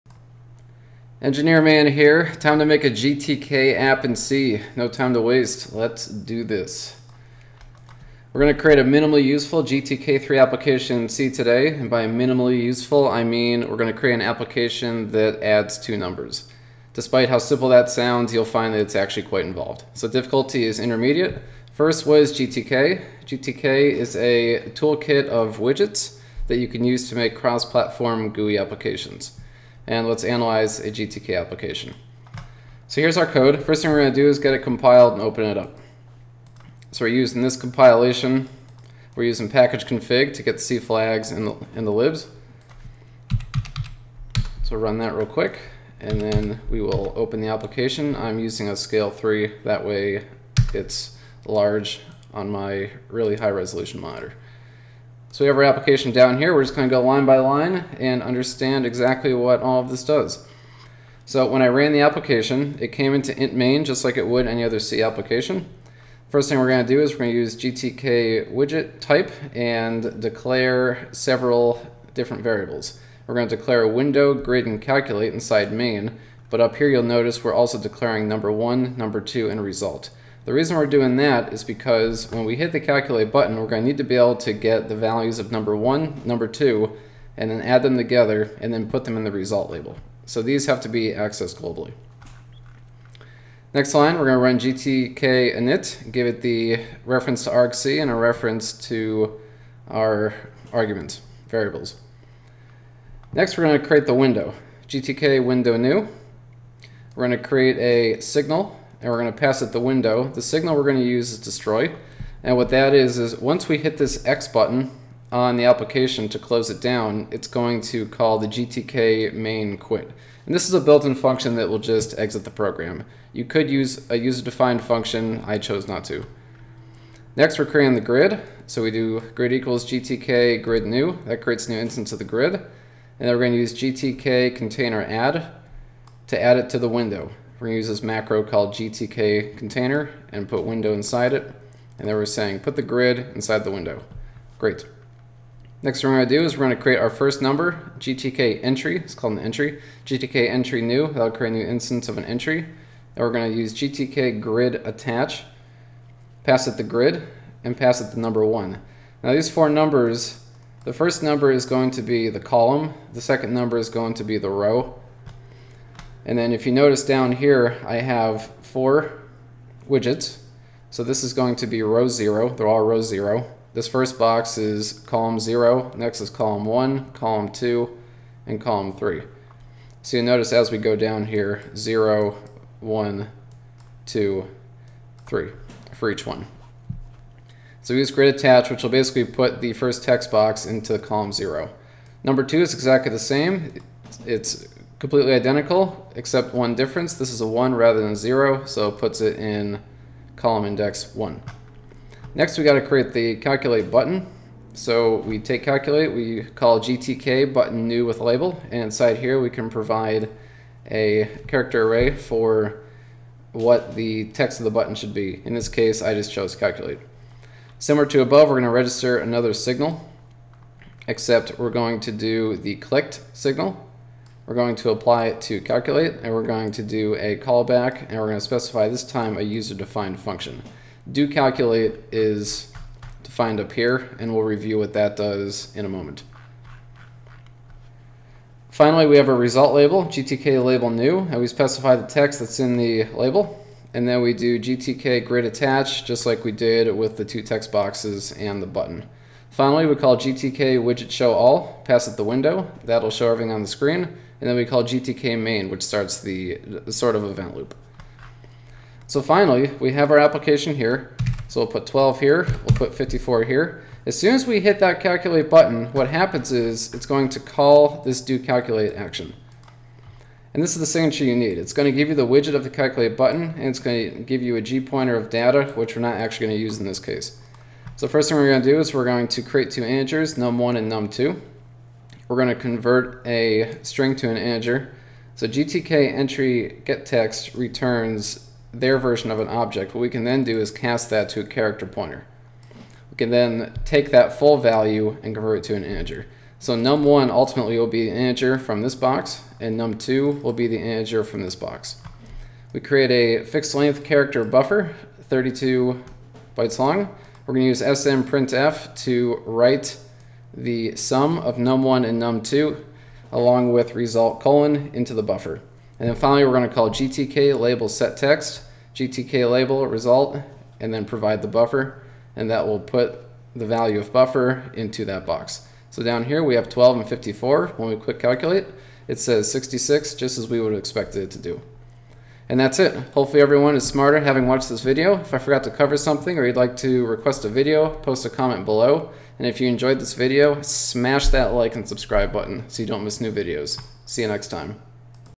Create a minimally useful GTK3 application in C
A quick video where we'll learn about GTK and analyze a basic GTK application that I wrote to add two numbers together.

Sincerely,
Engineer Man